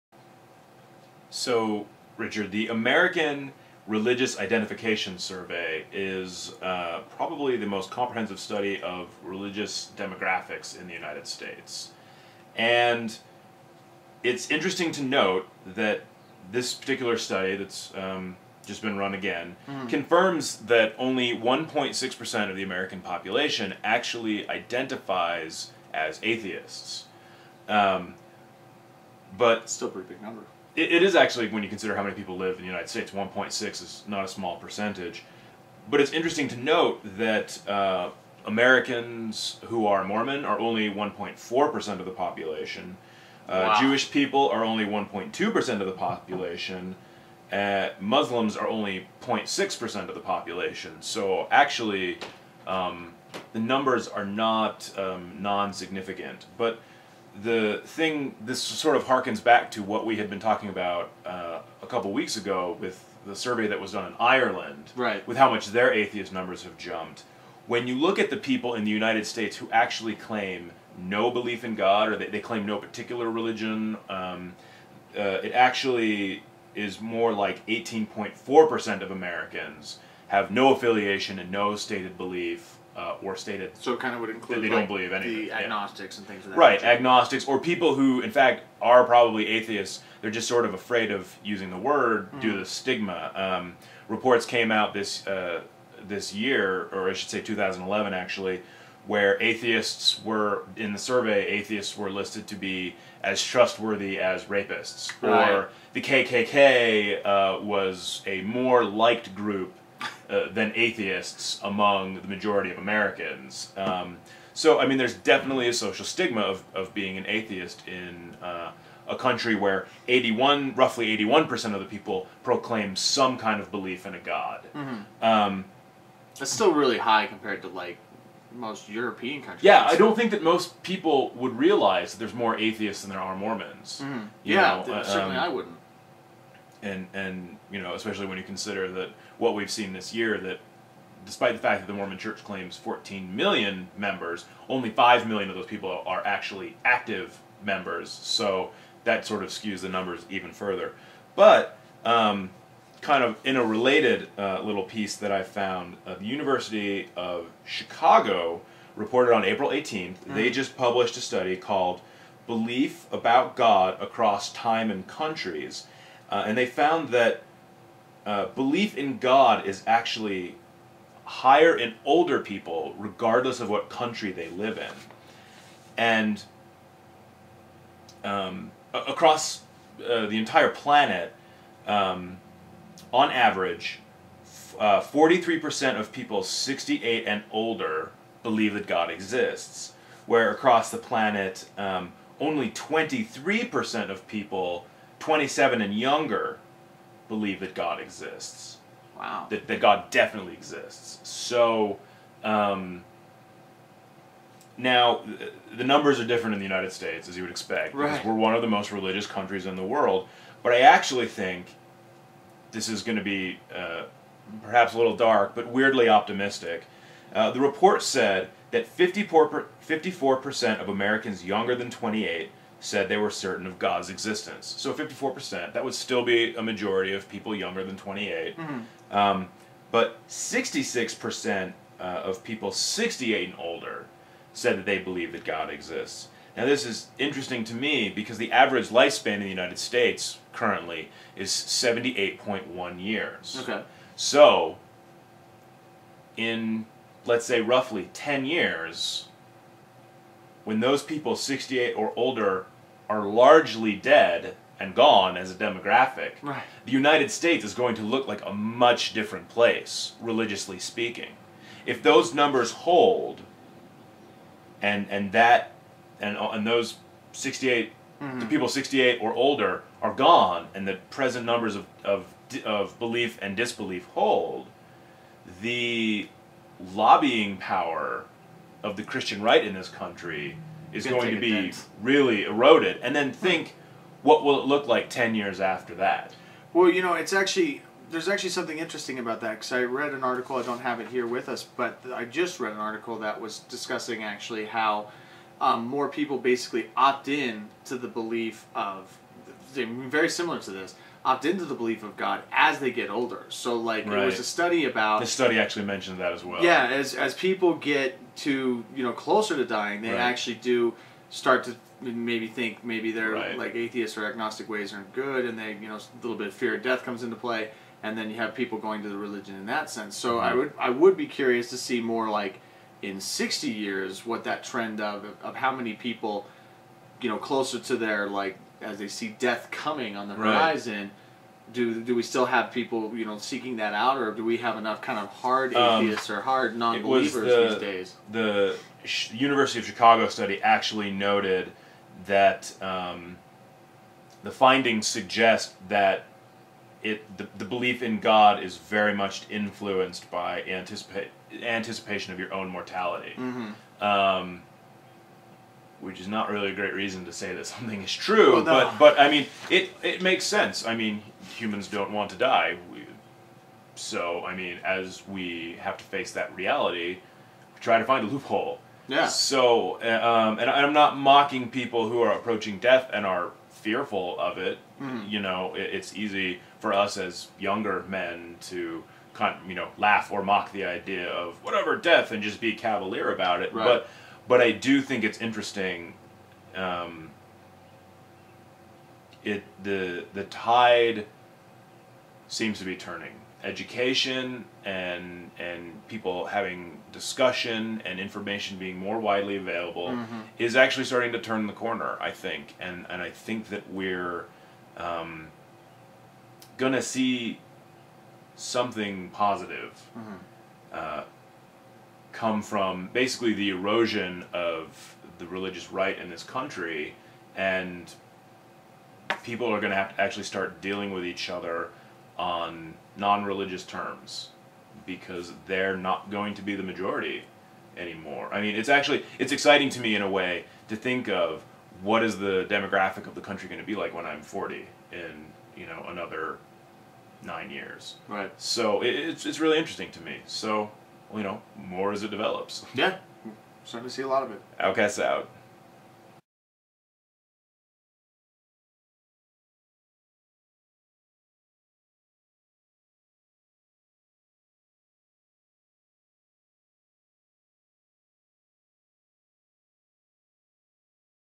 Belief in God Found in Decline
Belief in God is declining gradually worldwide, faith is highest among the older generations, and Atheists currently out number Mormon's in America.